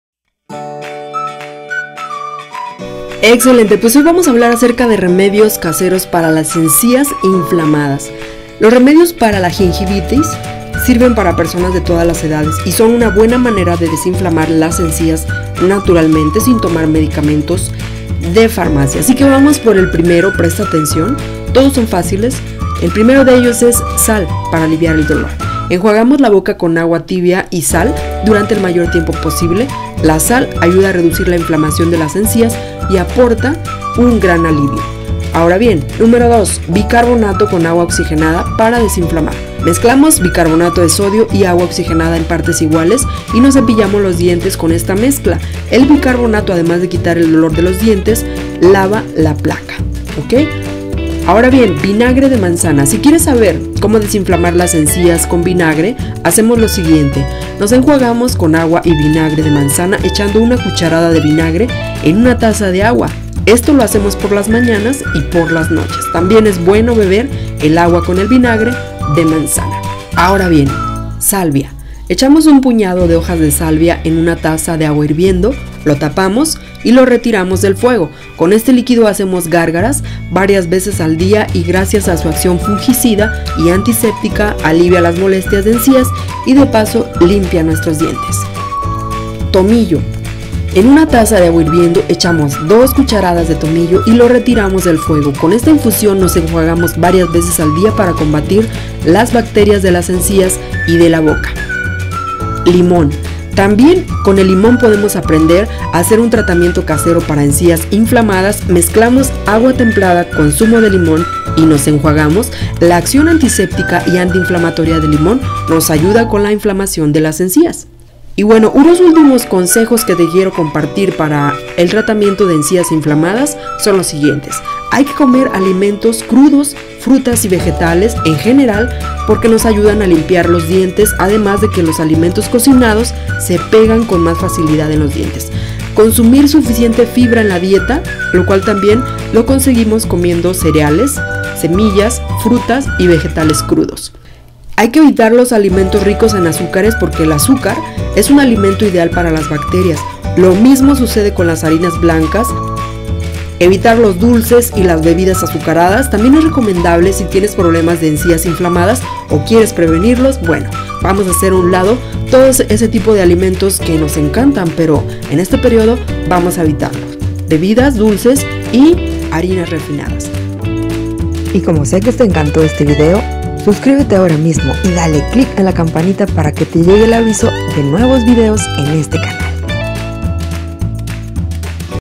✔ Remedio Casero para Encias Inflamadas
✔ Remedio Casero para Encias Inflamadas ( Sal, Bicarbonato, Vinagre )

Cuando piense en la salud dental, es probable que se concentre en prevenir las caries en los dientes. Pero también es importante prestar atención a las encías.

Las encías desempeñan un papel importante no solo en su salud dental, sino también en su bienestar general.

En muchos casos, las encías inflamadas y sangrantes son un signo de enfermedad de las encías.

Sin embargo, hay una serie de otras cosas que podrían estar causando problemas en las encías.

Cualquiera que sea la causa de las encías dolorosas y dolorosas, hay pasos que puede tomar para minimizar el daño y la incomodidad de las encías.

Gomas y técnica de cepillado.

En la búsqueda de mantener los dientes limpios, puede verse tentado a cepillarse los dientes tan vigorosamente como pueda. Sin embargo, las encías están hechas de tejido delicado, por lo que cepillarlas de forma incorrecta podría dañarlas.

Ya sea que opte por un cepillo de dientes manual o eléctrico, elija uno con cerdas suaves de nailon que tengan extremos romos.

Aunque puede encontrar cepillos con cerdas medias o duras, pueden dañar el esmalte de sus dientes o causar encías rojas e inflamadas.

Cuando se cepille, asegúrese de usar movimientos suaves y circulares para masajear y limpiar los dientes y las encías.

Si bien muchas personas usan un movimiento hacia adelante y hacia atrás, este movimiento puede irritar y dañar sus encías, causando que estén adoloridas y más propensas a sangrar o retroceder.

Janny Soltero